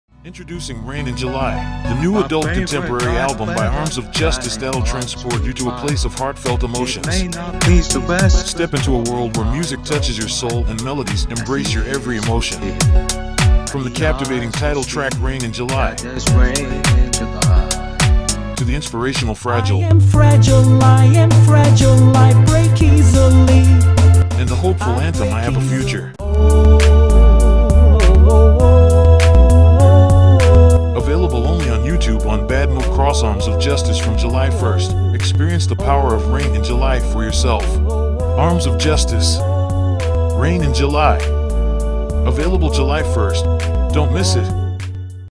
Rain In July. COMING JULY 1.
Rain In July
July 1

This is an amateur advert I made.
VOICEOVER: Davis
SCRIPT: ChatGPT
VIDEO CLIPS: Dezgo (Text-To-Video via Discord)